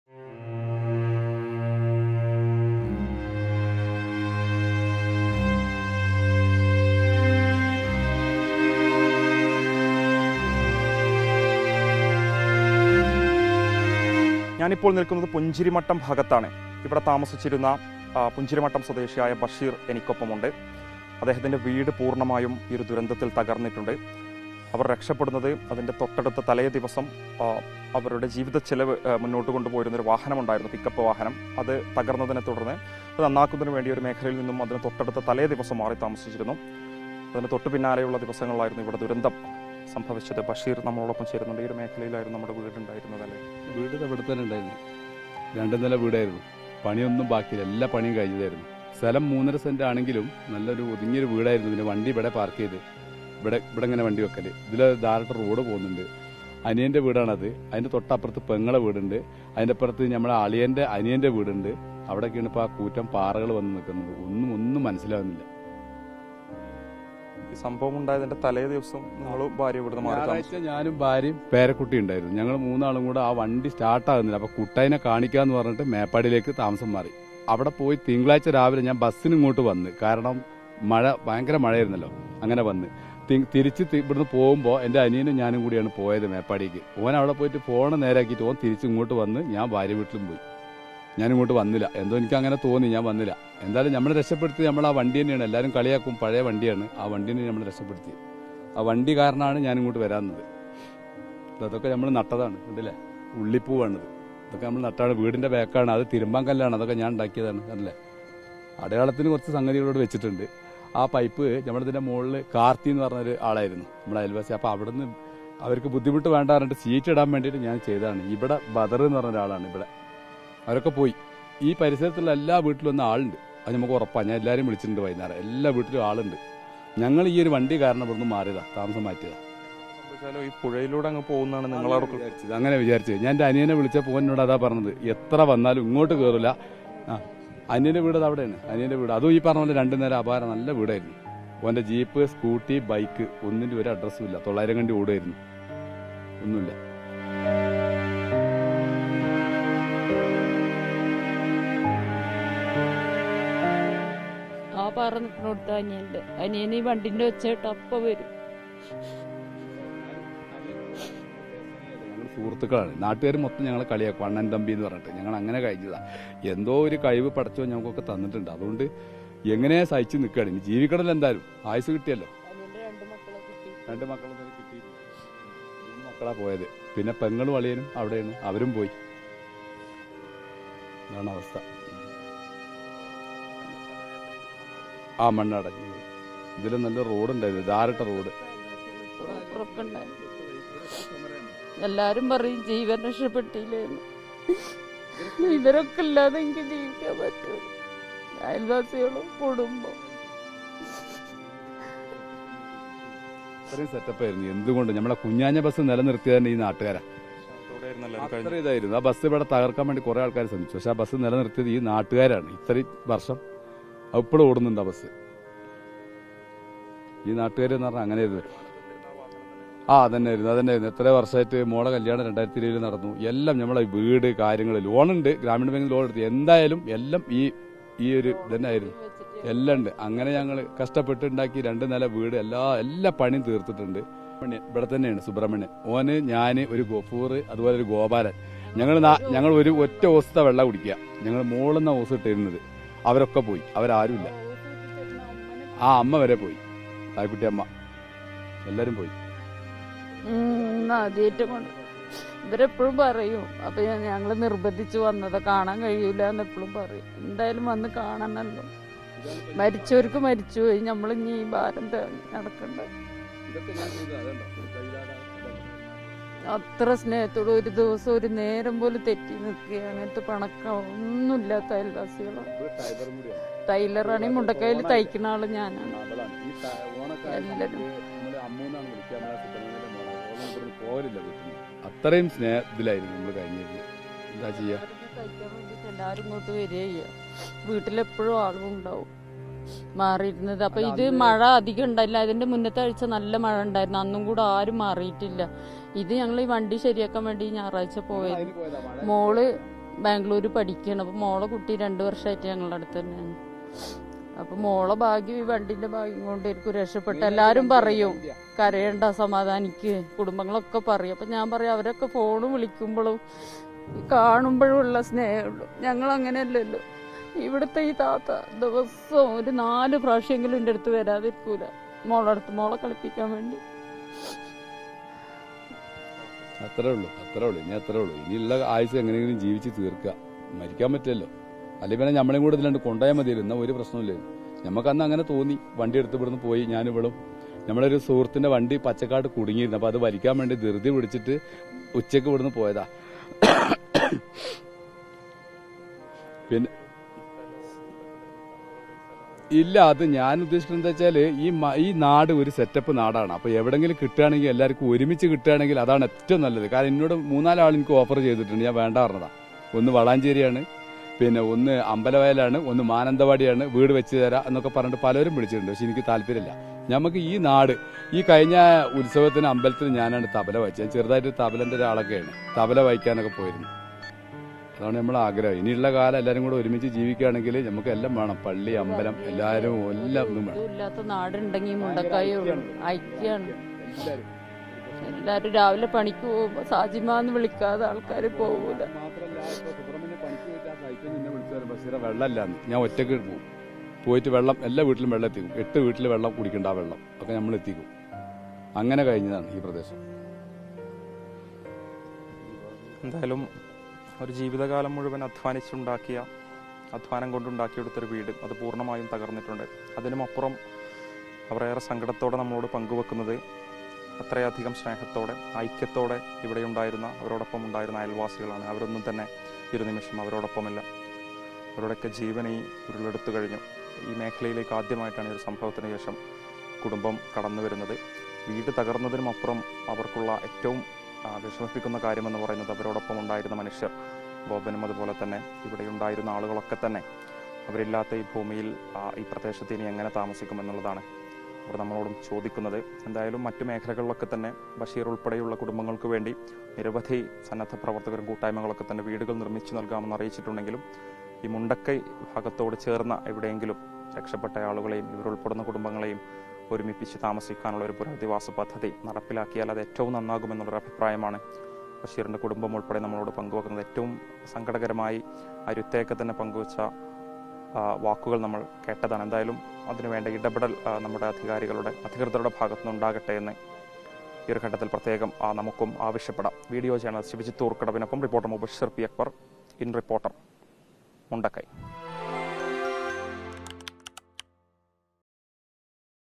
'പഴയതാണെന്ന് പറഞ്ഞ് എല്ലാവരും കളിയാക്കും, പക്ഷെ ആ വണ്ടിയാണ് ഞങ്ങളെ രക്ഷിച്ചത്' | Wayanad Landslide
'പഴയതാണെന്ന് പറഞ്ഞ് എല്ലാവരും കളിയാക്കും, പക്ഷെ ആ വണ്ടിയാണ് ഞങ്ങളെ രക്ഷിച്ചത്'; ഉരുൾപൊട്ടലിൽ നിന്ന് ബഷീറിനെയും, കുടുംബത്തെയും രക്ഷിച്ച ജീപ്പ്! | Wayanad Landslide

With Regards
Team RBC